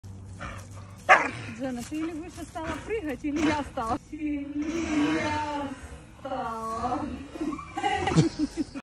JENA does not notice VIPER, but still gracefully jumps over ^_^
for the sake of playing with a stick, AmStaff JENA does not notice AmBulky puppy VIPER, but still gracefully jumps over it